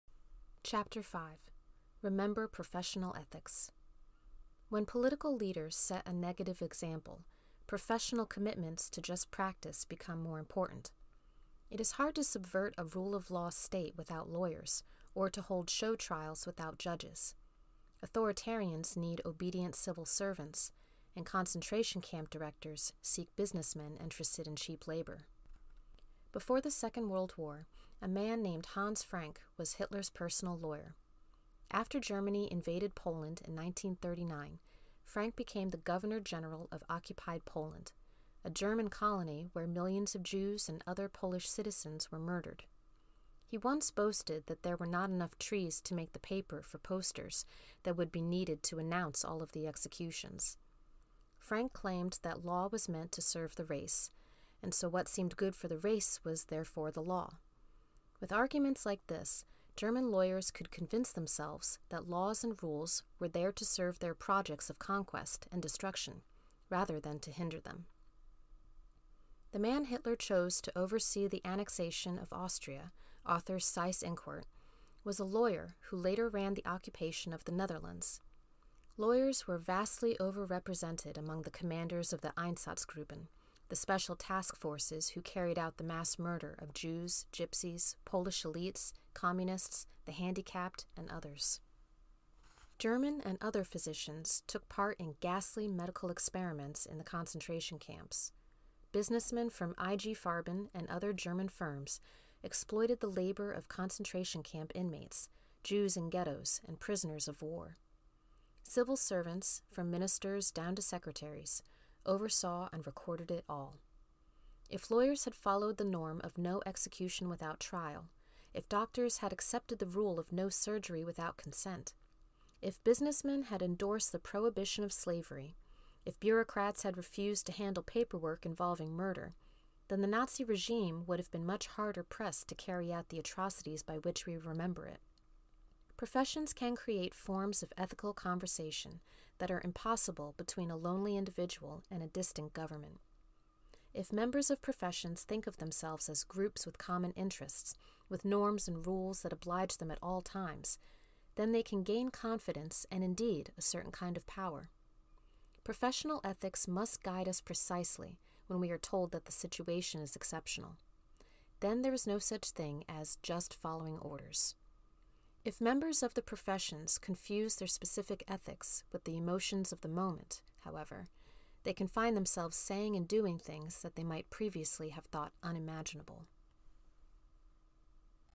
On Tyranny - Ch. 5 (audiobook)
The Founding Fathers tried to protect us from the threat they knew, the tyranny that overcame ancient democracy. Today, our political order faces new threats, not unlike the totalitarianism of the twentieth century. We are no wiser than the Europeans who saw democracy yield to fascism, Nazism, or communism. Our one advantage is that we might learn from their experience.

On Tyranny is a call to arms and a guide to resistance, with invaluable ideas for how we can preserve our freedoms in the uncertain years to come.

Narrated by EB